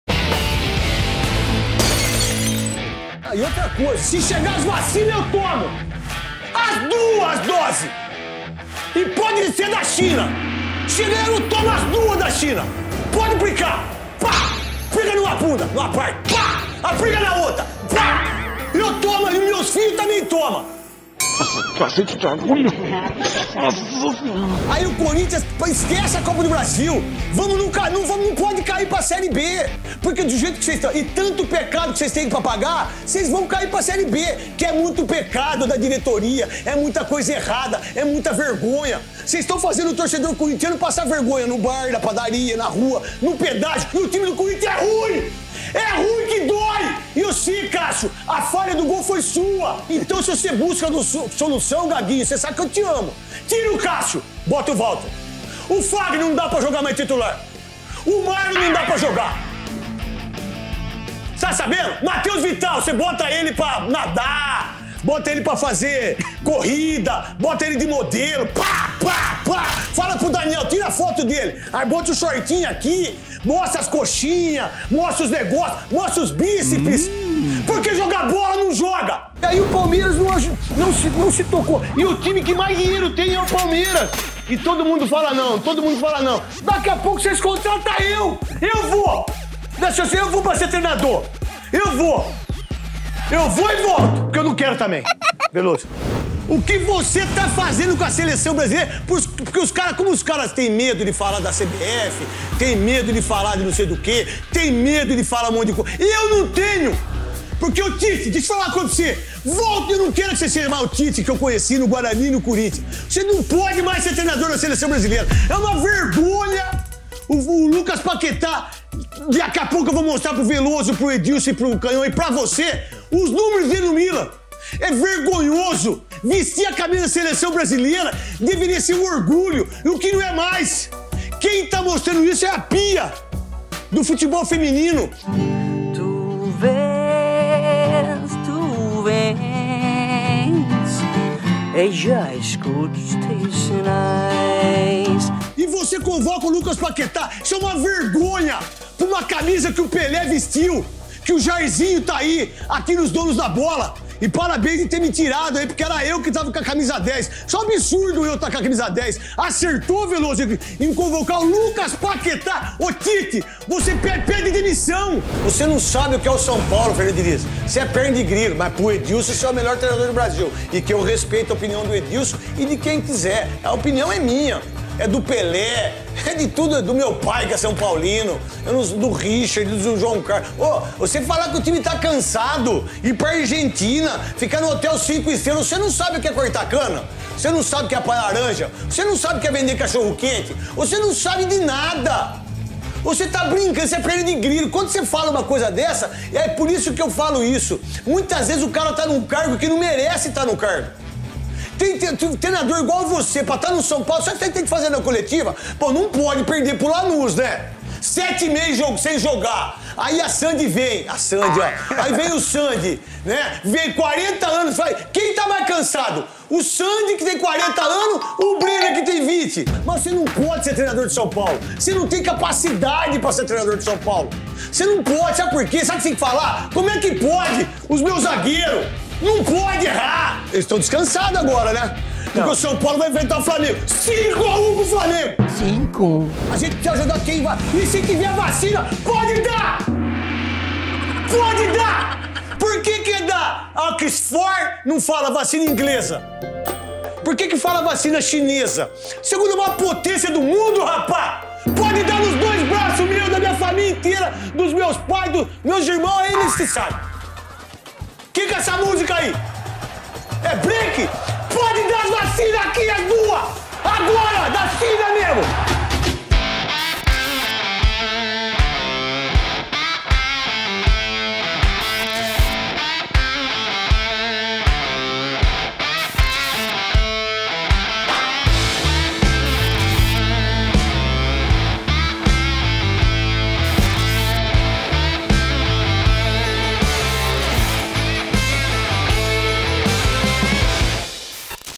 CRAQUE NETO PISTOLA COM TIME DO CORINTHIANS: RUIM QUE DÓI | NETO PISTOLA #29
Fala, garotinho. Já vi muito time ruim, mas igual esse do Corinthians, eu nunca vi. Não dá nem para assistir ao jogo, o torcedor já sabe que só vai passar raiva. É brincadeira.